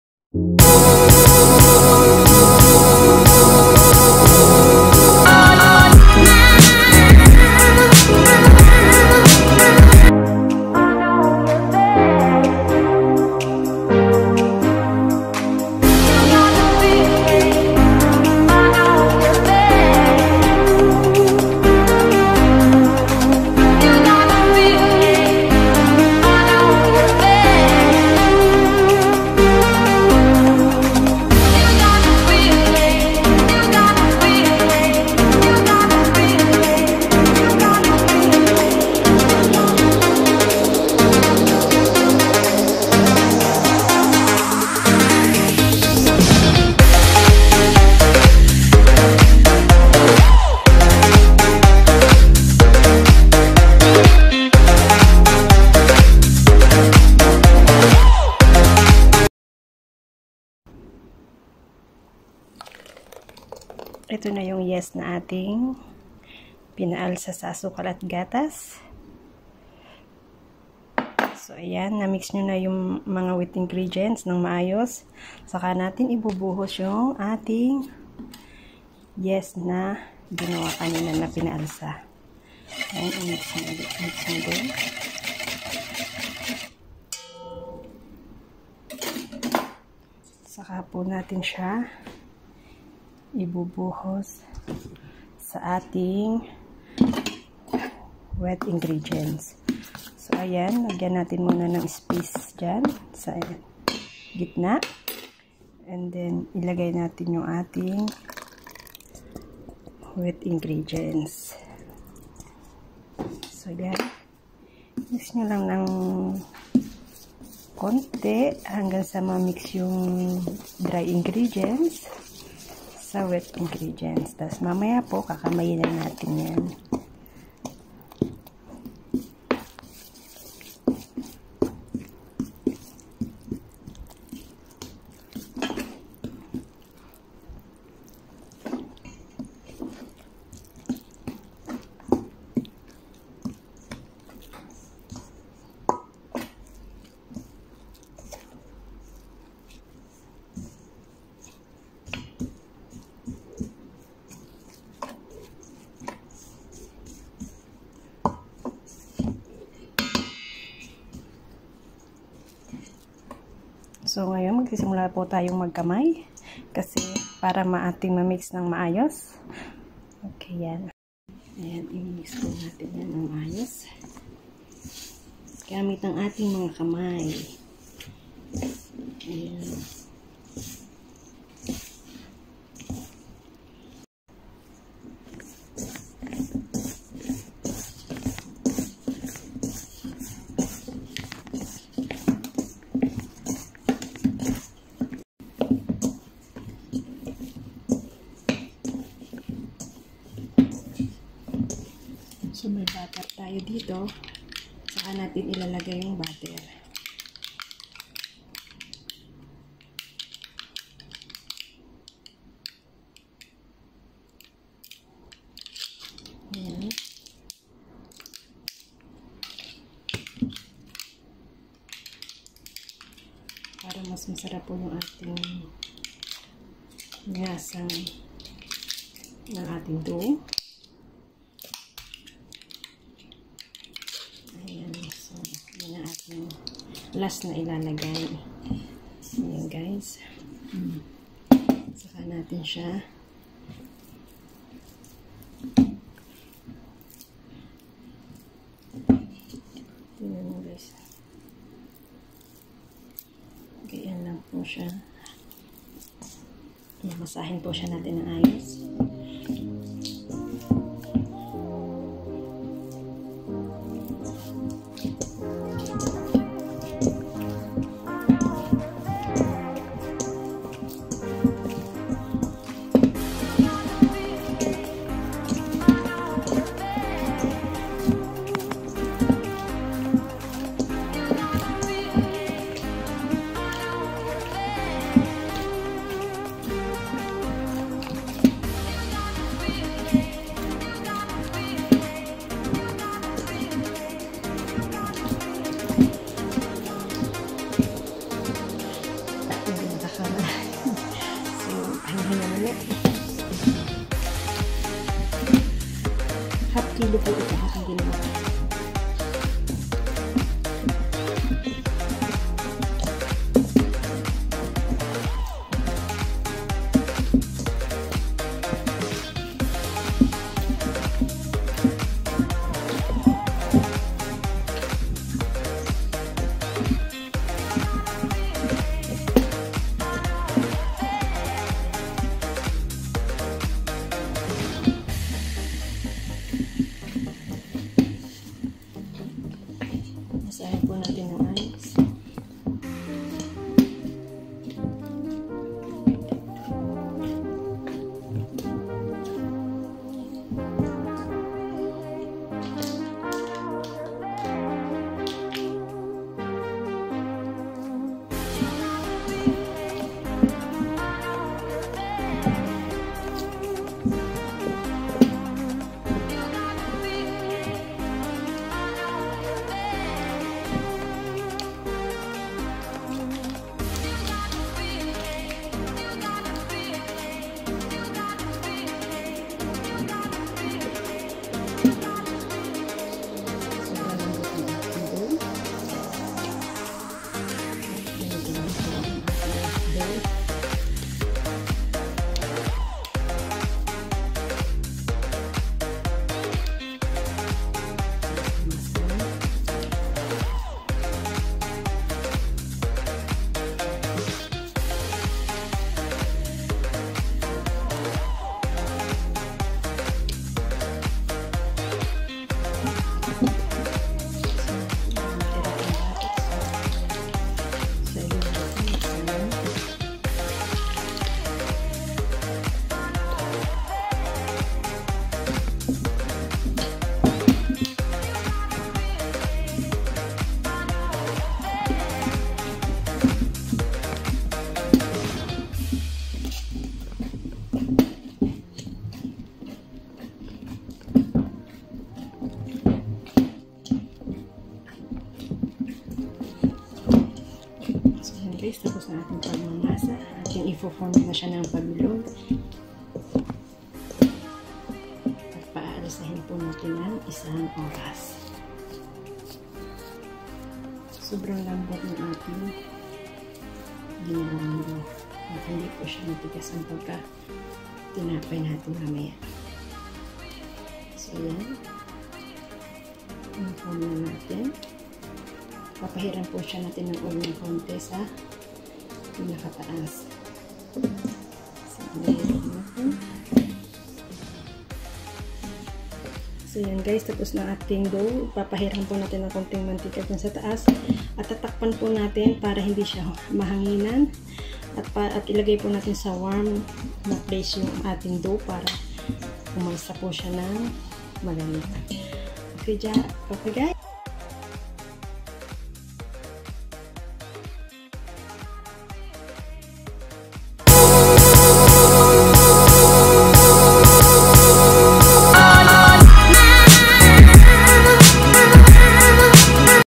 DOUGH /FATAYER/ PIZZA
Paraan kung paano gumawa ng napakadaling dough ng Fatayer at sa Pizza 😊♥️